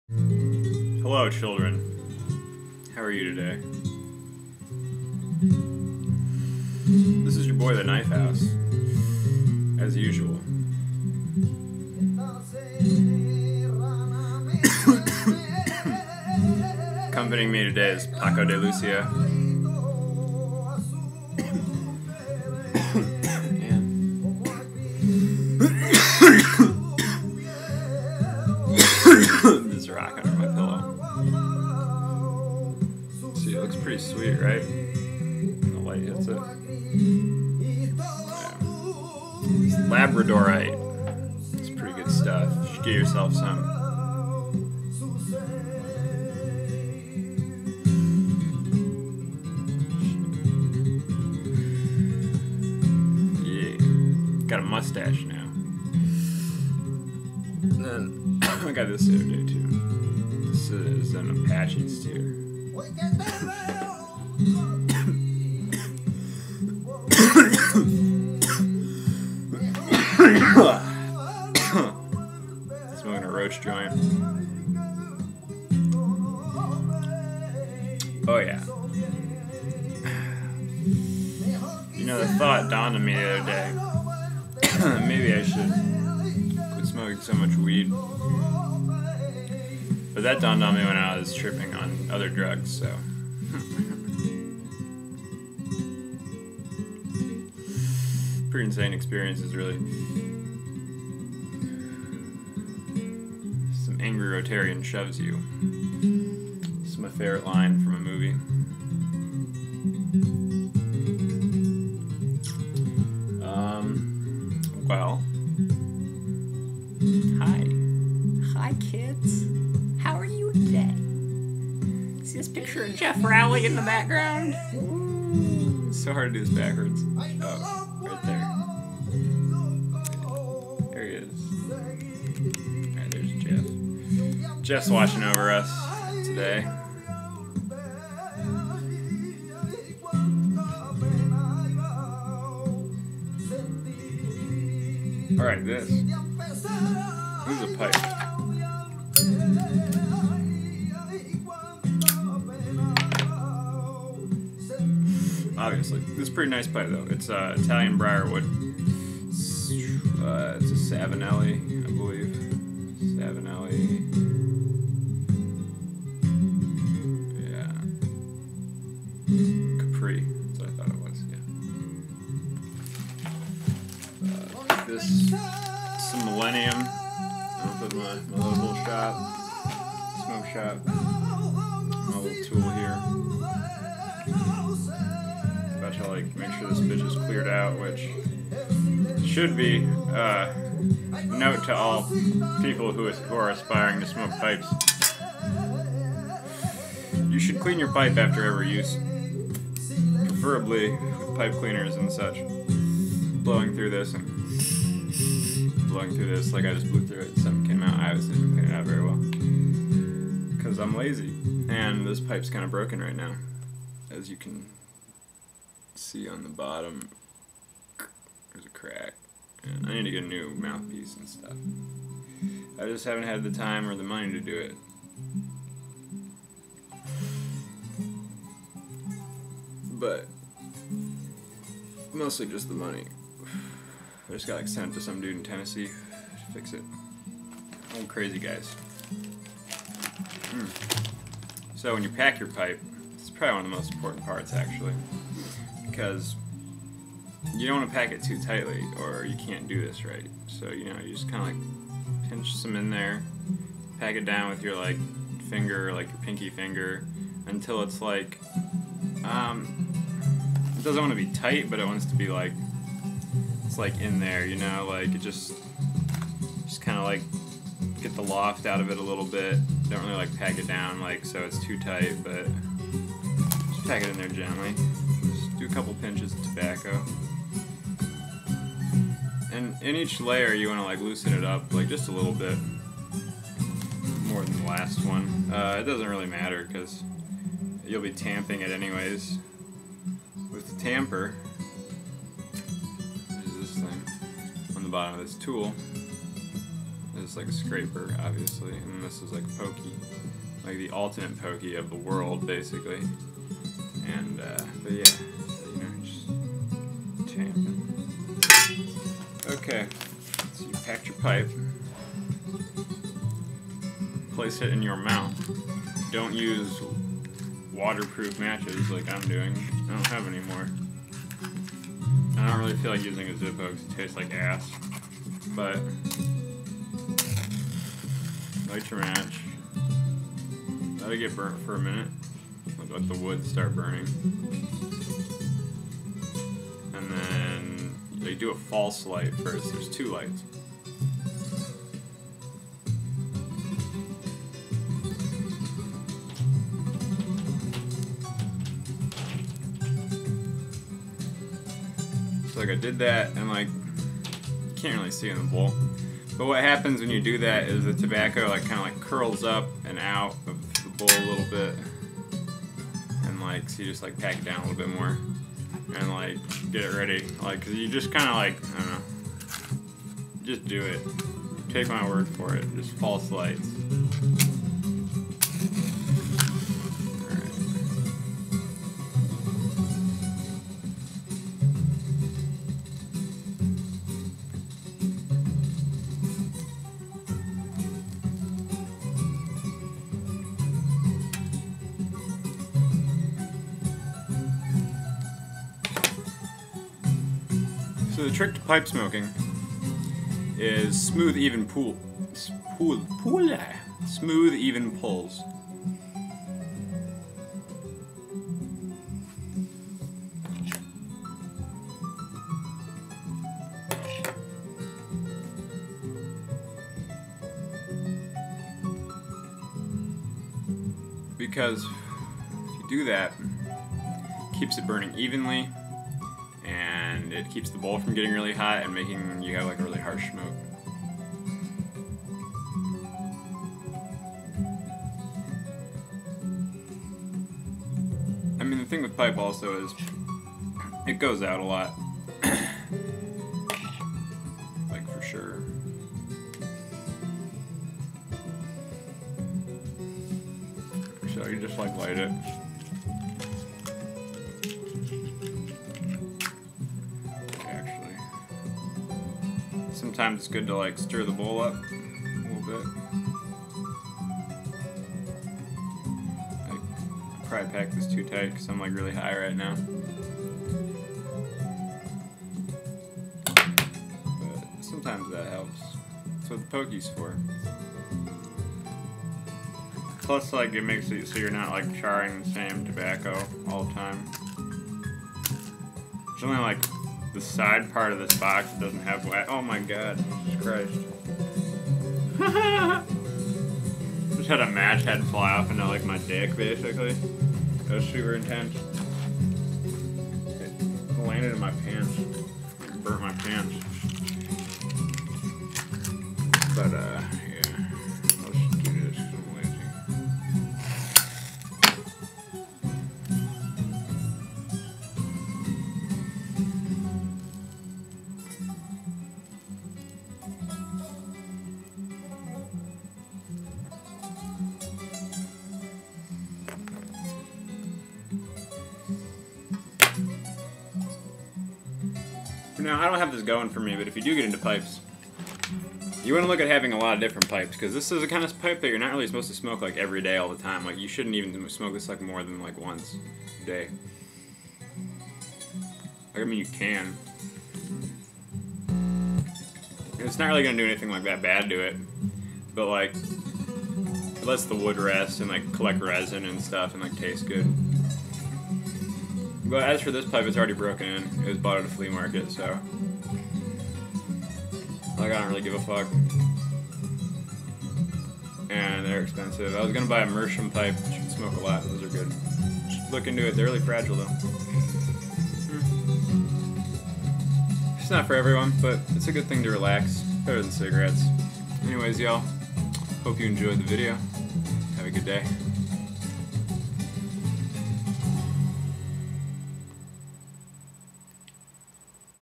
Intro Course To Pipe Smoking By The Knifehouse
Basics, good things, read more and talk to old men.